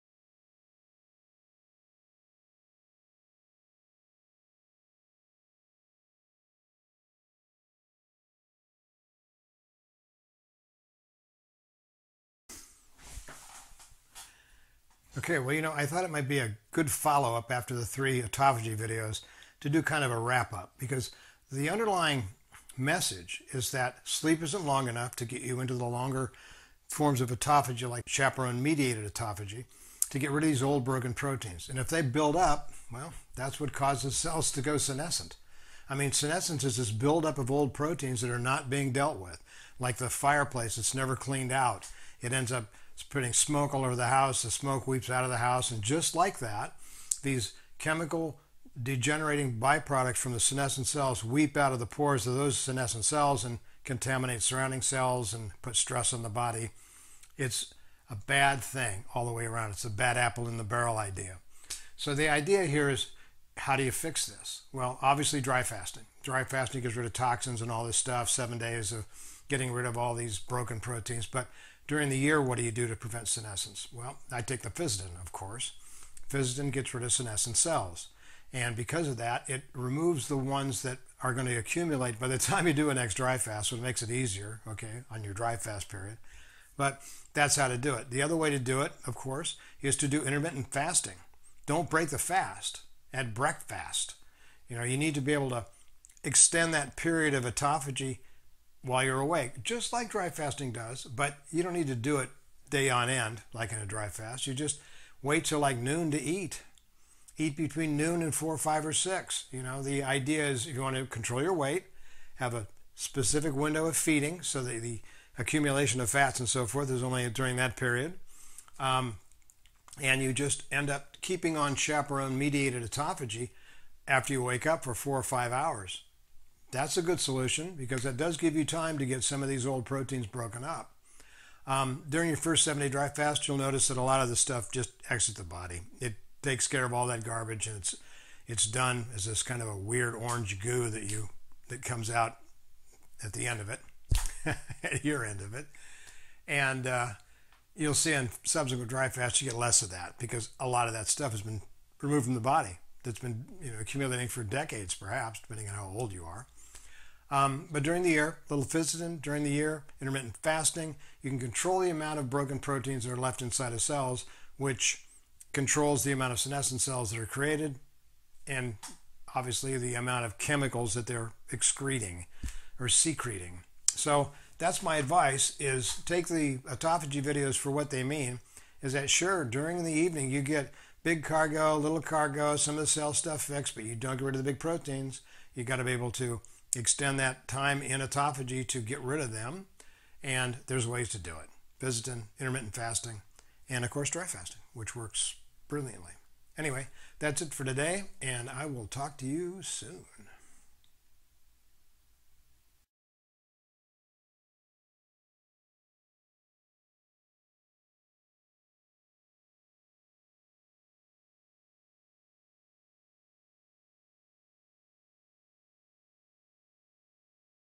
Autophagy and Senescence @LifeHacksChannel
Cellular autophagy is only as effective as you can keep it operation.  There are solutions!